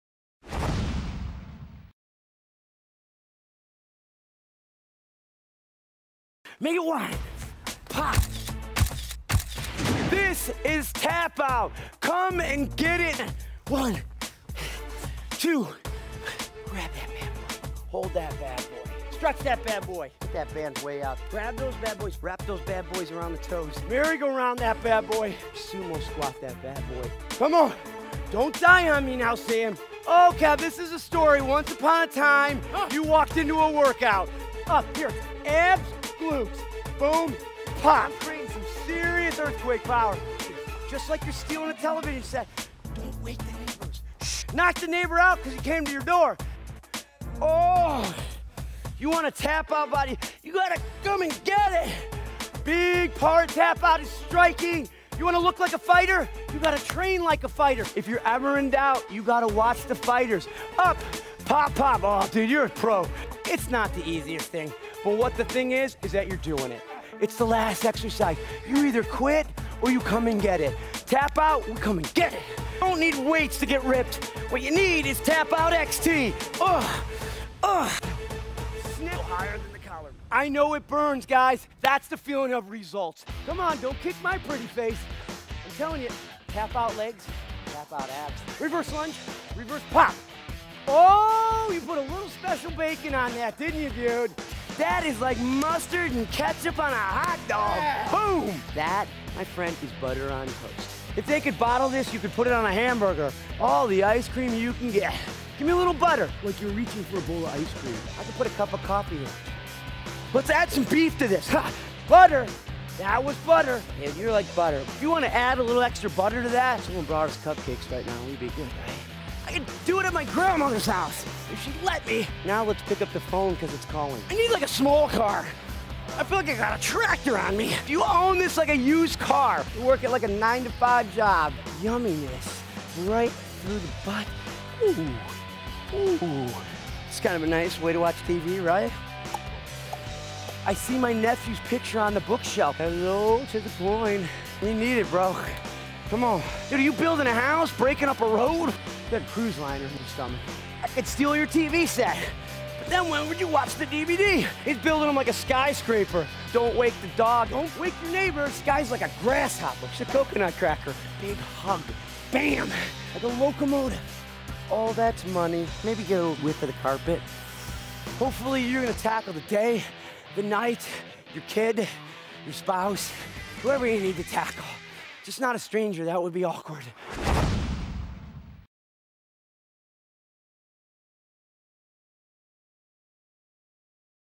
Best of TapouT XT Creator Mike Karpenko
Get to know TapouT XT creator Mike Karpenko and see how he motivates you to get the results you are looking for and push to the best shape of your life. Does TapouT XT Work- take a look at Mike and see the TapouT XT moves in action.